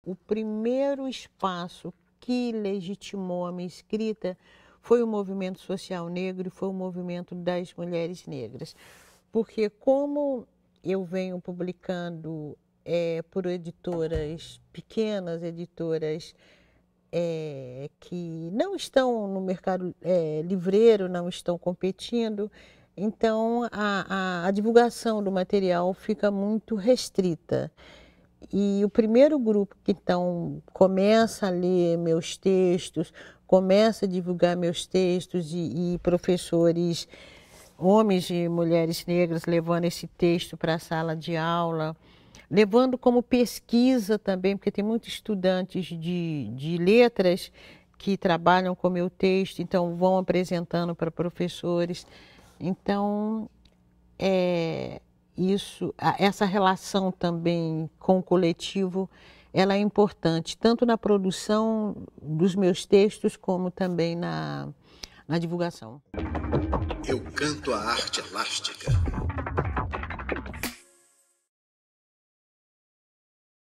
Conceição Evaristo conta como suas obras se tornaram uma referência da identidade negra
A escritora Conceição Evaristo estreou na literatura com o romance Ponciá Vicêncio, que narra as alegrias e tristezas vividas por uma mulher negra. Segundo a autora, por ter sido lançado em uma editora pequena, o livro não tinha tanto espaço nas livrarias. Apesar disto, Evaristo conta que o seu primeiro romance conquistou leitores através da divulgação do movimento de mulheres negras.
A autora também comemora o fato do livro Olhos d'água ter se tornado referência em diversas provas de vestibular. Segundo Evaristo, isto se deve à lei 10.639/2003, que incluiu no currículo escolar a obrigatoriedade da temática "História e Cultura Afro-Brasileira", dando espaço para escritores que representam a identidade negra.
Conheça a trajetória literária de Dia 29 de agosto às 21h30, com reprise na madrugada de segunda para terça às 3h30 e de sábado para domingo à 0h30.